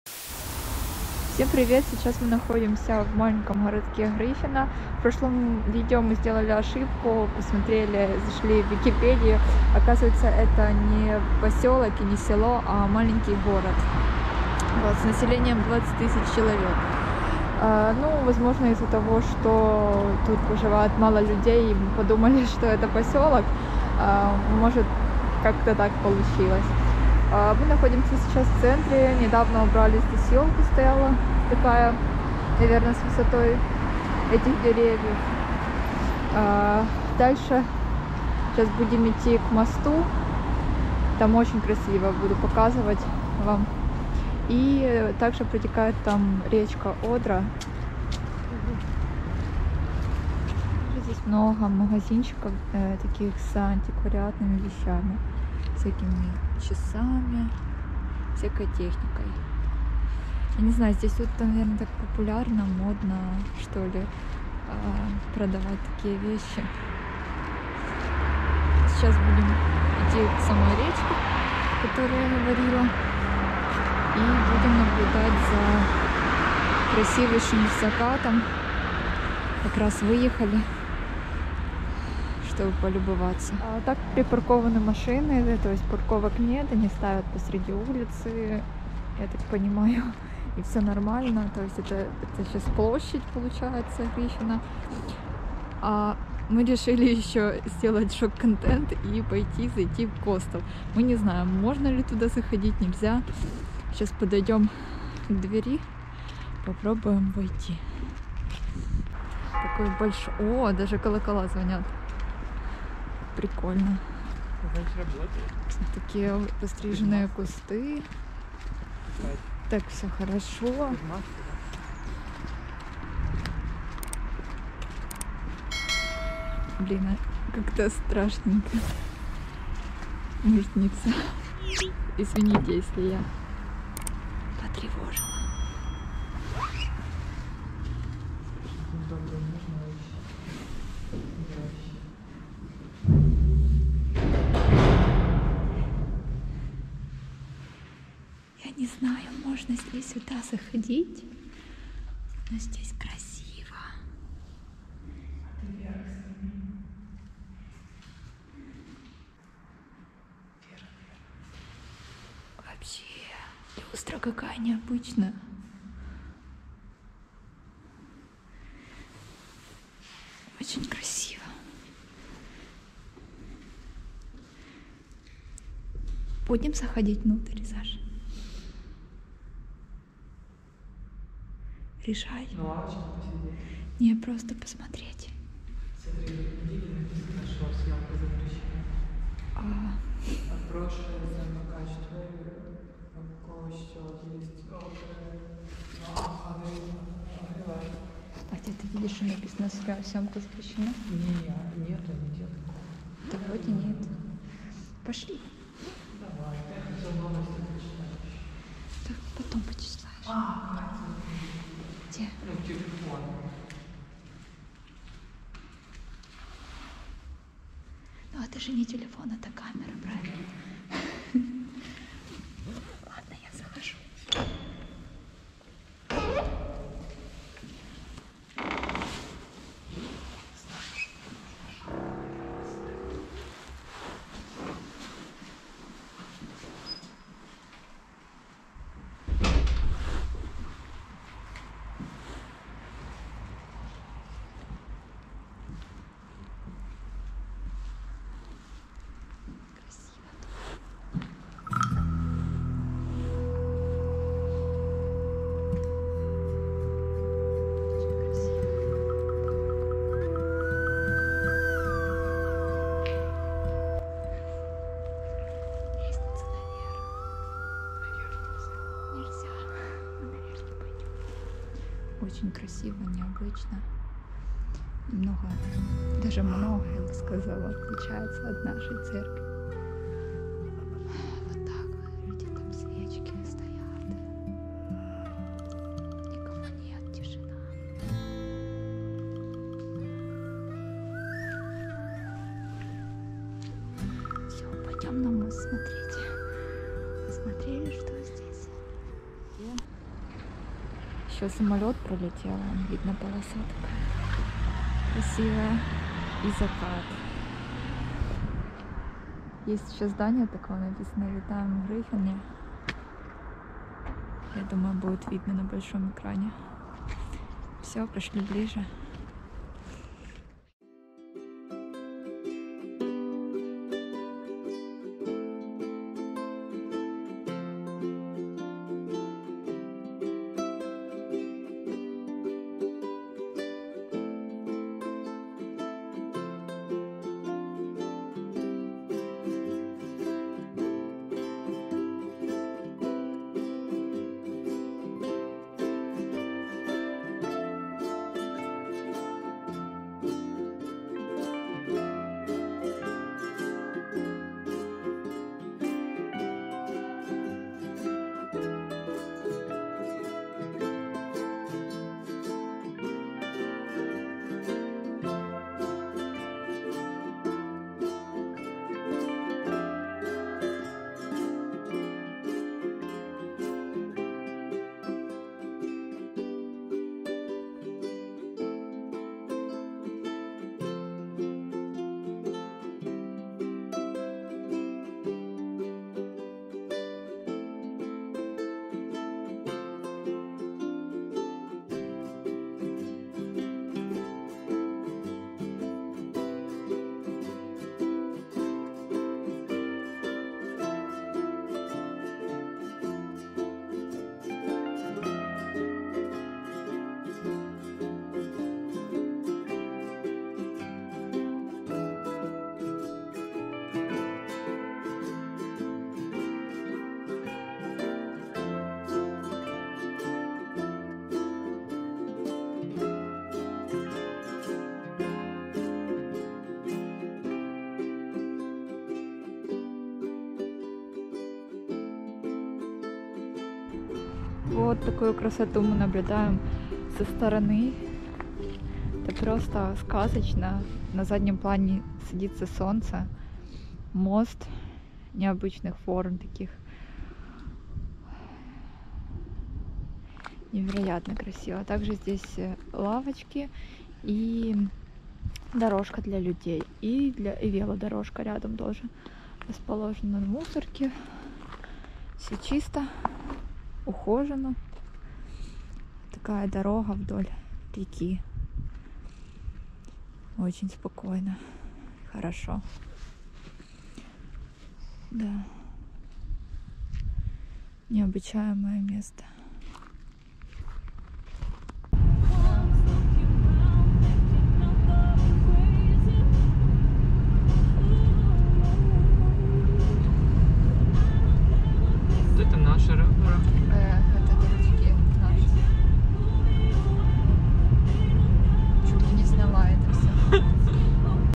Прогулянка вулицями Польщі, Gryfino |Костел Virgin | Міст
Грифіно (Gryfino), до 1945 Грайфенхаген (нім. Greifenhagen) - місто в Польщі, в 20 км на південь від Щецина, в 3 км від річки Одера і кордону з Німеччиною.

   Населення становить 21 000 . У місті є підприємства хімічної, легкої, харчової промисловості. Через Грифіно проходить залізниця Щецин-Костшин-над-Одрою.

Місто є туристичною базою для двох природних парків – Долини Нижньої Одри та Букової пущі.